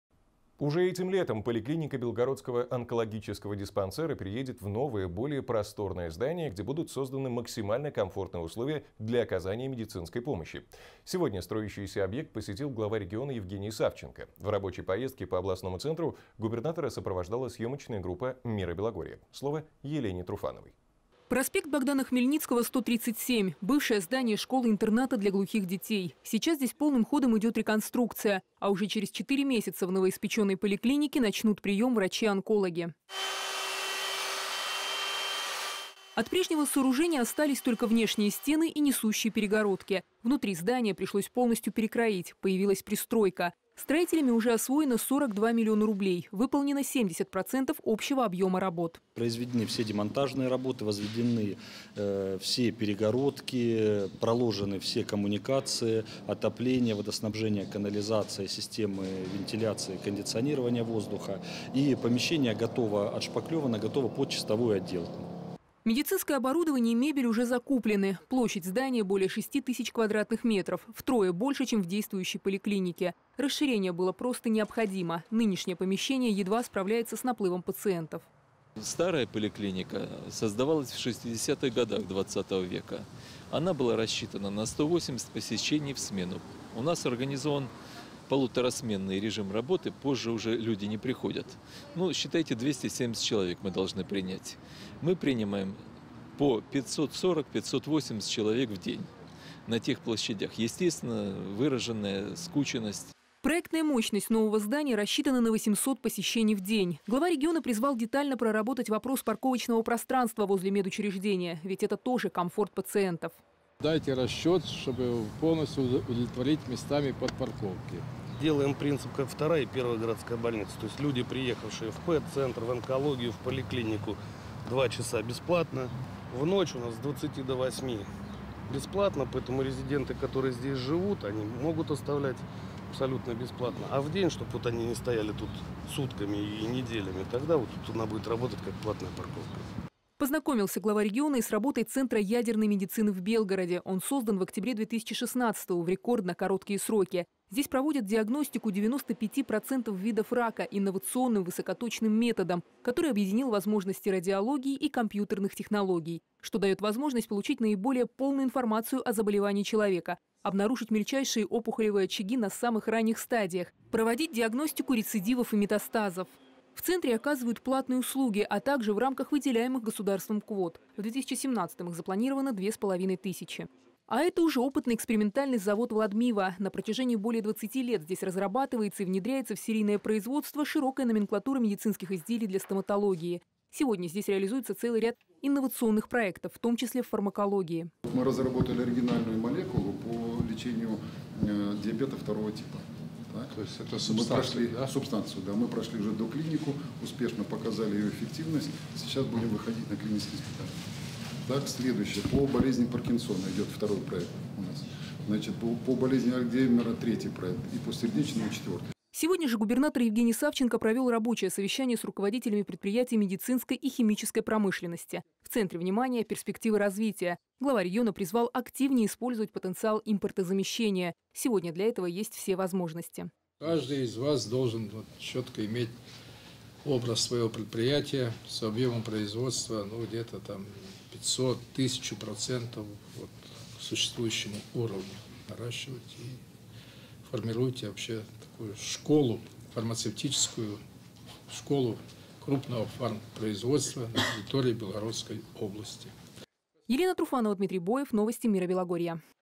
Новое здание для онкодиспансера
Уже этим летом поликлиника Белгородского онкологического диспансера переедет в новое, более просторное здание, где будут созданы максимально комфортные условия для оказания медицинской помощи. Сегодня строящийся объект посетил лично глава региона Евгений Савченко. В рабочей поездке по областному центру губернатора сопровождала съемочная группа «Мира Белогорья». Слово – Елене Труфановой.